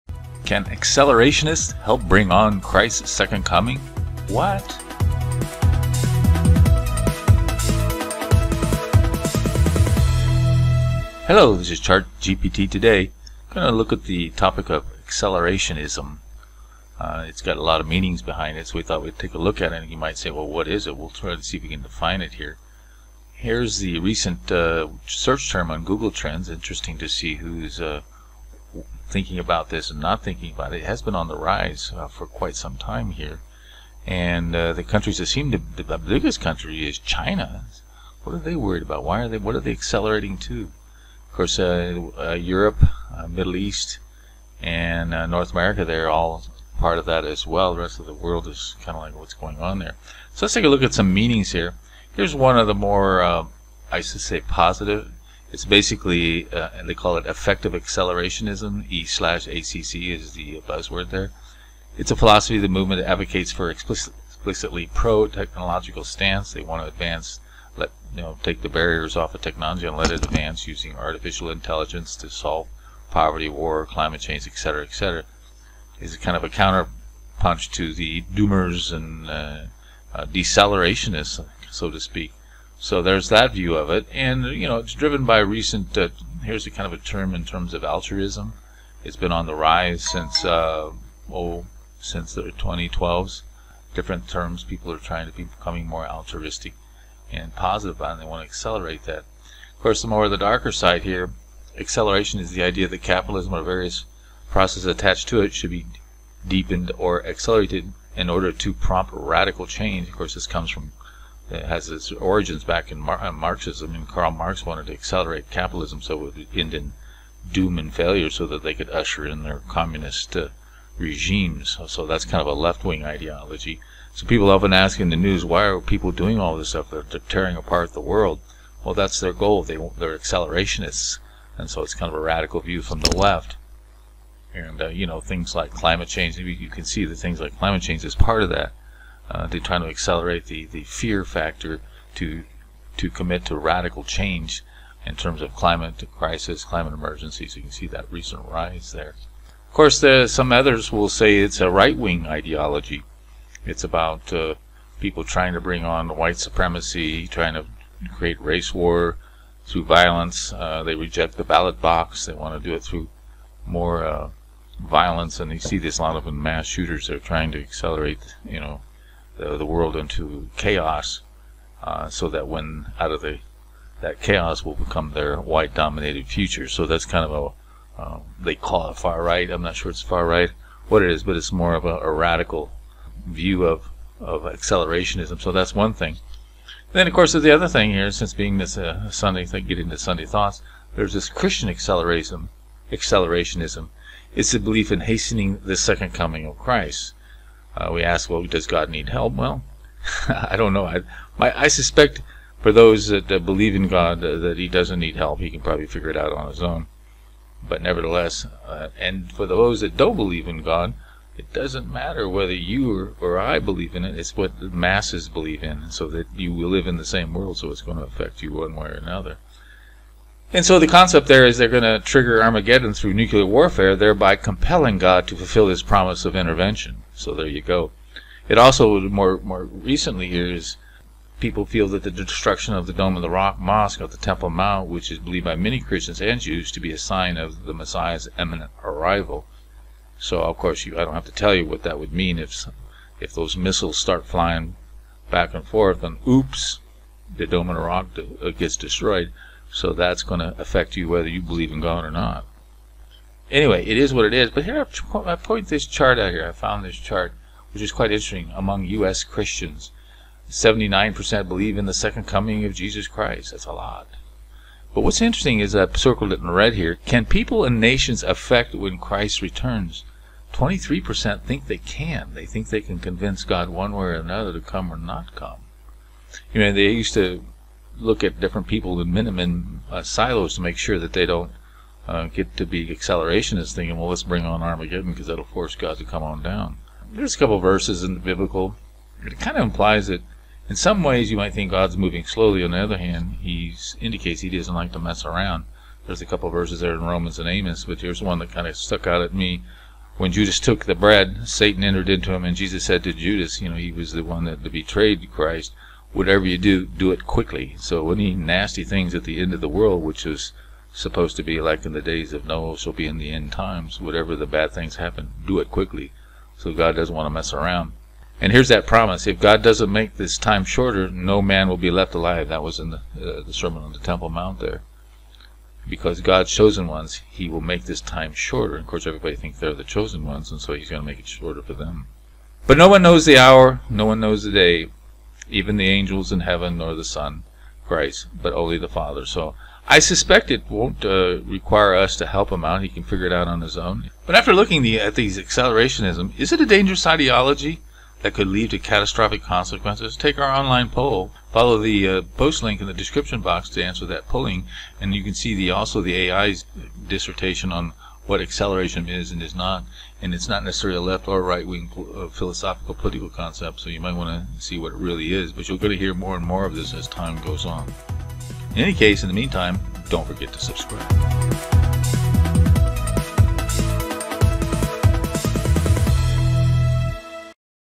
Can Accelerationists Help Bring On Christ's Second Comming?
Accelerationism is a concept that proposes accelerating or exacerbating existing trends with the belief that this acceleration will lead to desirable changes. Dangerous?